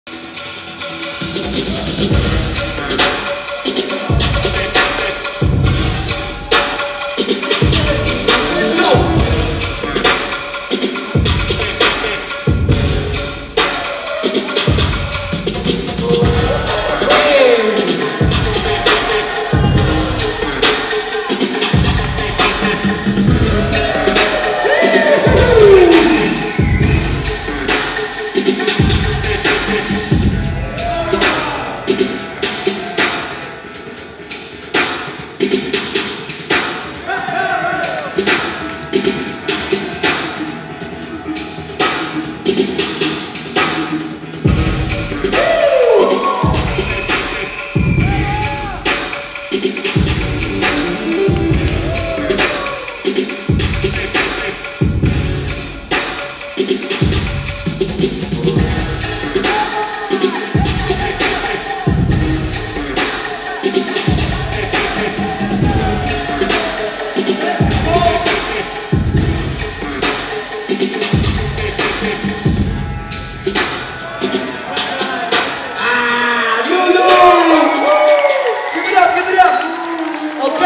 Luulu | Move For Freedom Festival 2019
At the Move For Freedom Festival 2019, organised on the August 16 & 17 in Geisenheim, Germany.

Judge showcase by Luulu

Hosted by Maxim Seth
Video by Roschkov.media